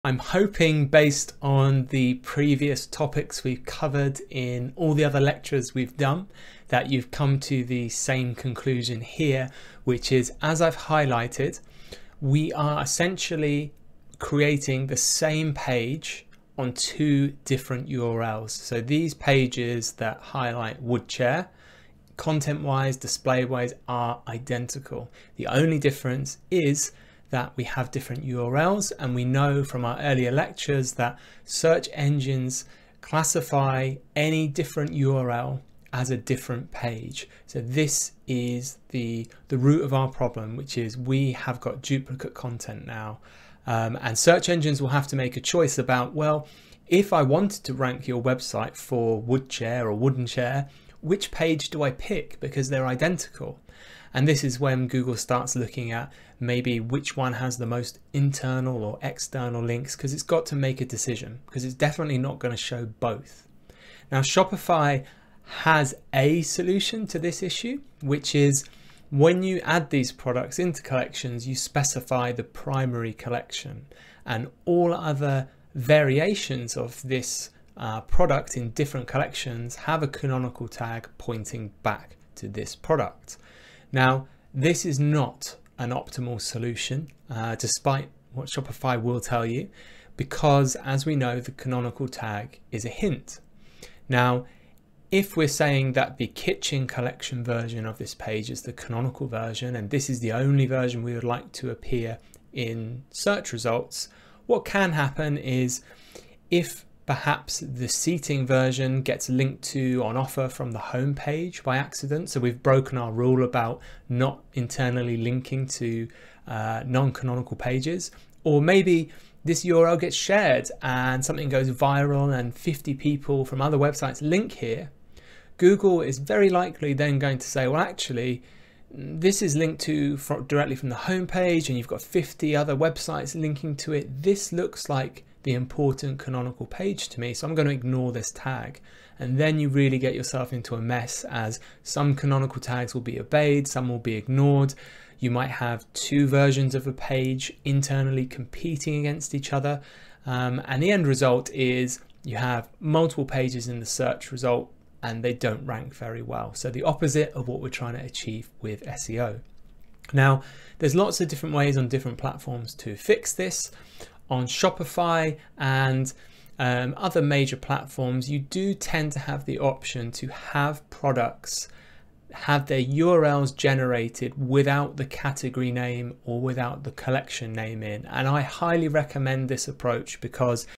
Canonical tag issues with Shopify
In this lecture you'll see how the Shopify platform can cause issues with canonical URLs.